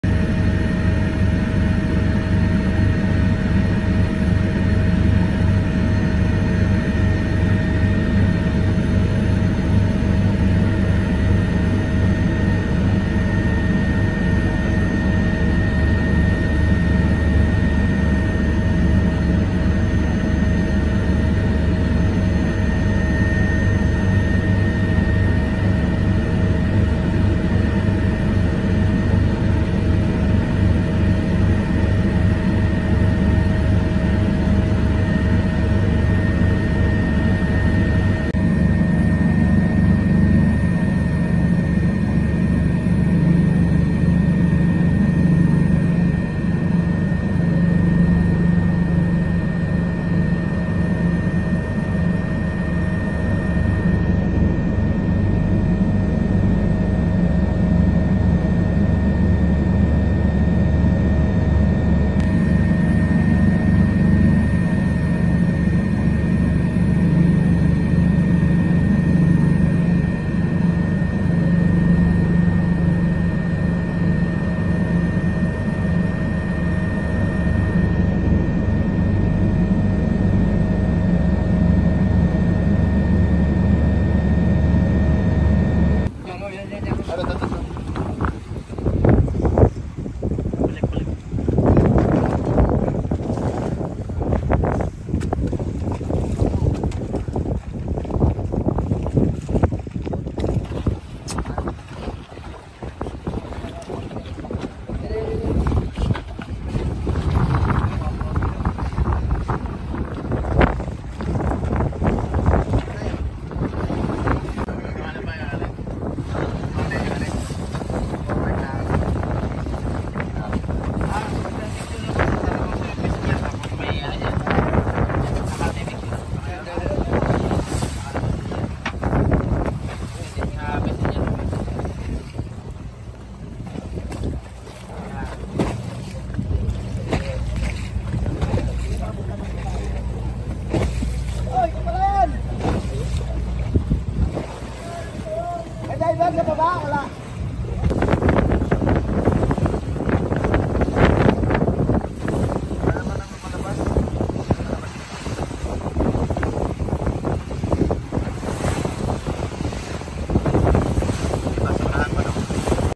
PCG observes 'very minimal' oil sheen along Manila shoreline | ABS-CBN News
The Philippine Coast Guard (PCG) observed "very minimal and unnoticeable oil sheens" in the shorelines of Manila, Bulacan, Pampanga, Bataan, and Cavite after conducting an aerial survey on Tuesday.